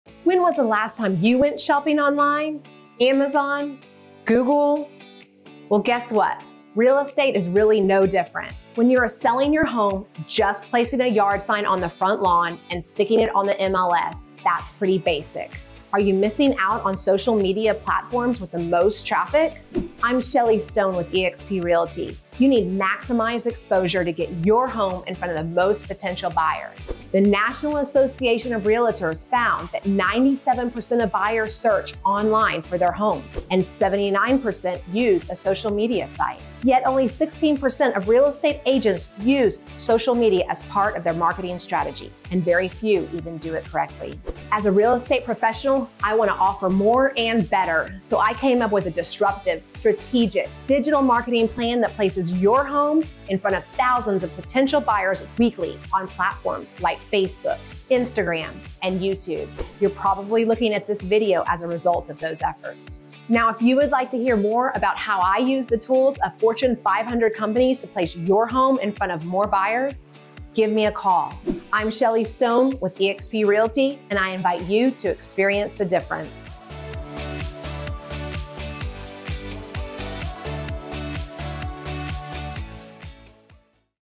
You'll Get More Money Selling Your Home Doing This One Thing
You'll Get More Money Selling Your Home Doing This One Thing; Marketing! If you don't have digital marketing as you put your home on the market, you will wait for your home to sell. Selling a home requires experience, marketing expertise, and strategic investments to get your home in front of the right audience.
So, what all goes into digital marketing in Houston real estate? Let's explore it together.

Shelley Stone
The Shelley Stone Group @ eXp Realty